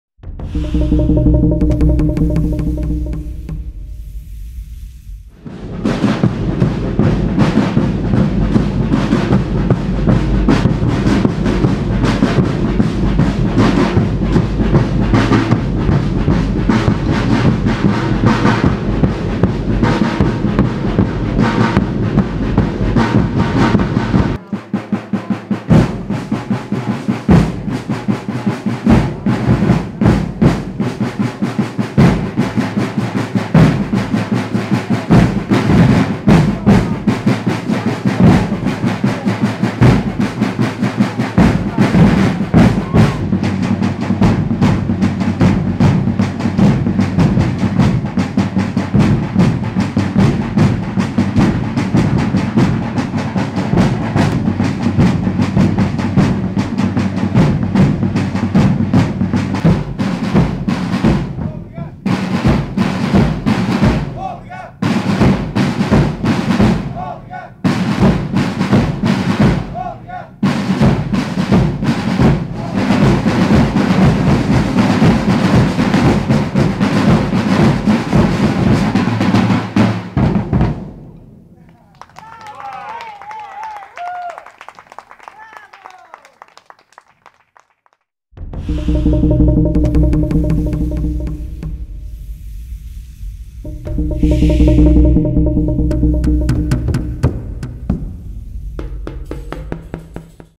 Palcos do Românico | Retumbar dos Muros | Drumming GP
5 julho 2014 | Igreja de Tabuado | Marco de Canaveses

Espetáculo integrado no programa cultural "Palcos do Românico", promovido no âmbito do Programa de Dinamização Cultural e Turística da Rota do Românico.